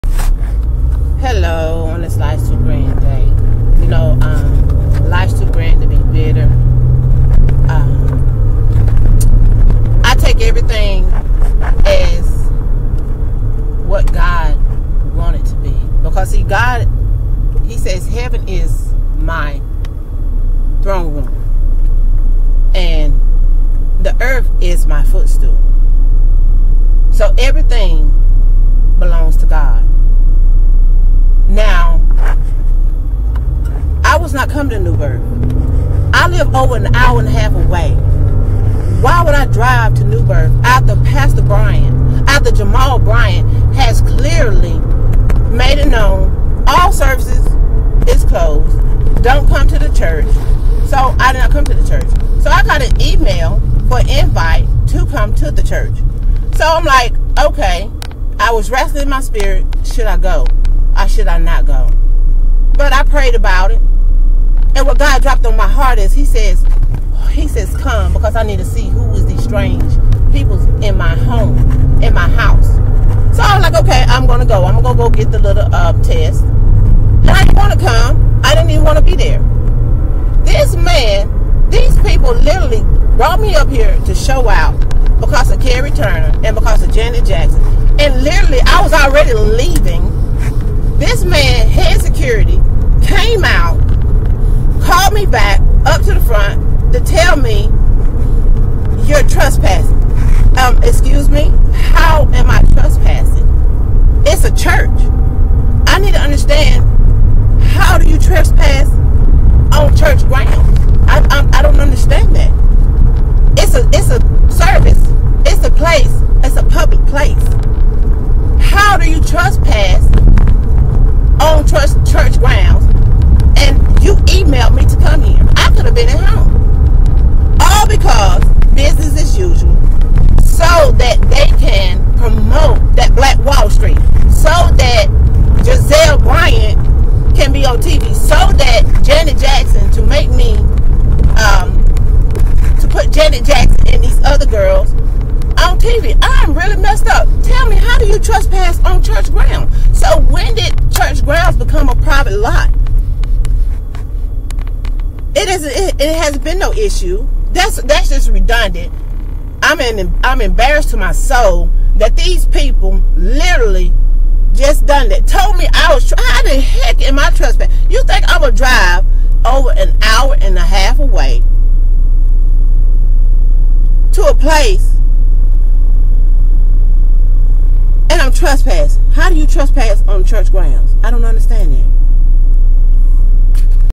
Kingdom Sends Greeting The Audacity of New Birth Church, After They emailed me to come to the church
This ignorant security guard had the audacity to say "your trespassing, your no longer allowed on the promise but you can come to the food bank" you how much of an idiot you sound like. first off how are you trespassing on a church grounds"
The White House paying Pastor Bryant to Have me kicked out of a church I didn't want to come too.